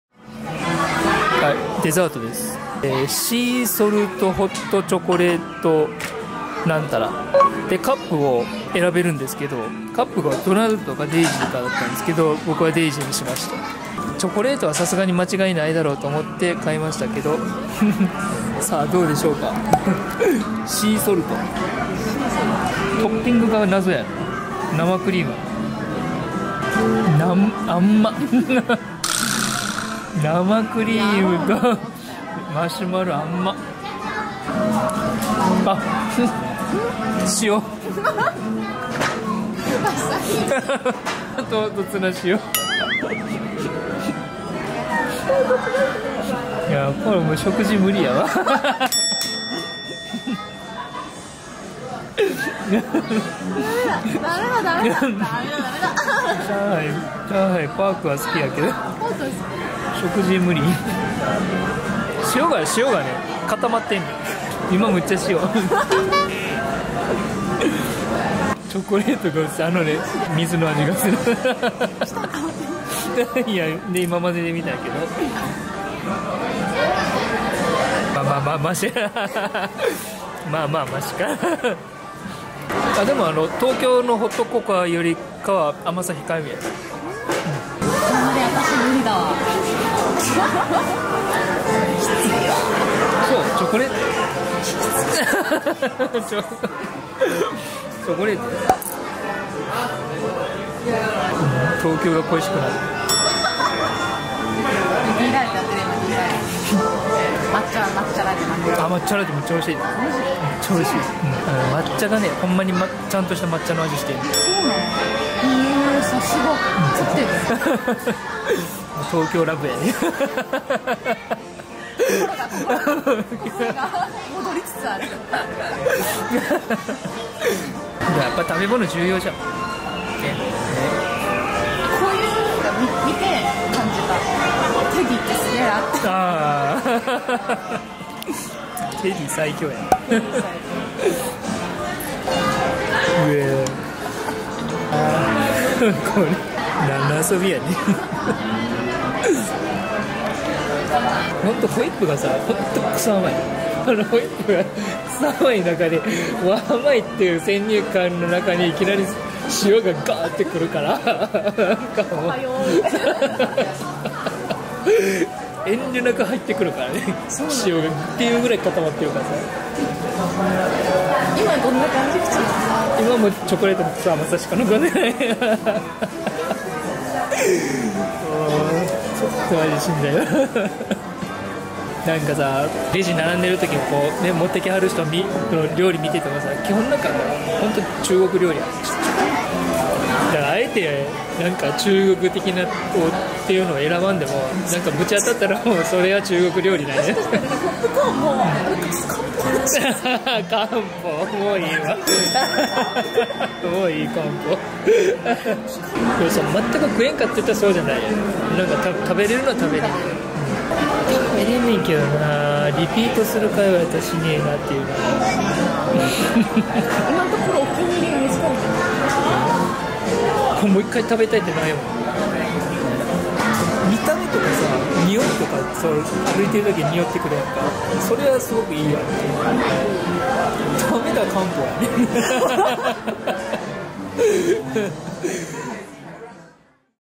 【上海の試練２】甘辛のメリハリパンチが強いホットチョコレート（Tangled Tree Tavern 2018年11月23日 SHDL）
このお店の近くにラプンツェルの塔があったんですね。で、周辺を見回しても塔の近くへ行ける道が無くて…。「この店の中から行けるんじゃね？」ってことで入ったのが「Tangled Tree Tavern」というお店でした。

入ったらからには何かを注文しなきゃならないのですが、ちょうどお昼ご飯を食べた後（ ）だったので、何かデザート的なものをということでシーソルトなホットチョコレートを頼んでみました。

しかしまぁ、なんというかクソ甘ｗ。じゃっかん水っぽい感じもするし。しかも塩が混ざって無くて時々、高濃度塩分の直撃を喰らいます。いやほんと、やっぱ上海パークの食事はムリでした…。

デイジーのスーベニアカップはカワイイです。